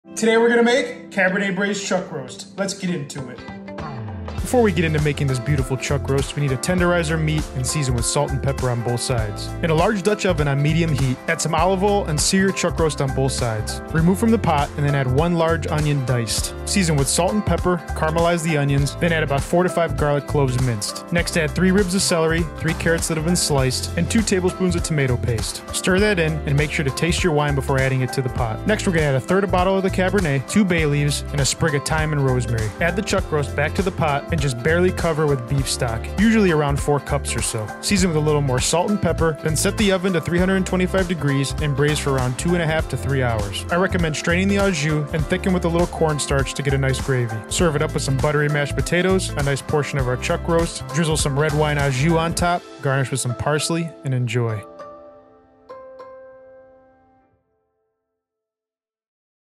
You Have To Try This Cabernet Braised Chuck Roast!
Cabernet Braised Beef Chuck Roast

Ingredients:

5-6 Lb Beef Chuck Roast
1/2 Bottle Cabernet Sauvignon
1 Onion (diced)
3 Carrots (sliced)
3 Celery Ribs (sliced)
4-5 Garlic Cloves
2 Tbsp Tomato Paste
2 Bay Leaves
2 Sprigs Thyme
1 Sprig Rosemary
1 qt Beef Stock
1 Tbsp Corn Starch
Olive oil

Instructions:

*Generously season chuck roast with olive oil, salt and pepper.

*In a Dutch oven or braising vessel on medium/high heat, add 2-3 Tbsp of olive oil. Sear the chuck roast for about 3-5 minutes per side to get a good Maillard reaction and color. Remove the roast from the Dutch oven and set aside.

*In the same Dutch oven reduce the heat to medium then add onion, salt and pepper. Caramelize onions slightly then add celery, carrots, and garlic. Sauté for 5 minutes.

*Next add tomato paste to the bottom of the pot and once it starts to simmer, stir into the vegetables.

*Add red wine, bay leaves, thyme, and Rosemary. Bring to a simmer. Add the seared roast back to the pot then add beef stock making sure the stock covers at least 3/4 way up on the roast. Season with a little more salt and pepper.

*Cover with a lid and braise in the oven set to 325 degrees Fahrenheit for about 2.5-3 hours. Every 45 minutes turn the roast so it cooks evenly. Once done poke the roast  with a fork or skewer and if it goes in like butter it is done and tender.

*Strain the au jus into a separate sauce pan and bring to a gentle simmer. In a small bowl combine 1 Tbsp a corn starch and 2 Tbsp of cold water. Whisk and make a slurry. Add slurry very little at a time to the simmering au jus and whisk until desired thickness.

*Serve with mashed potatoes or polenta and a vegetable on the side. Top with some of that delicious red wine au jus.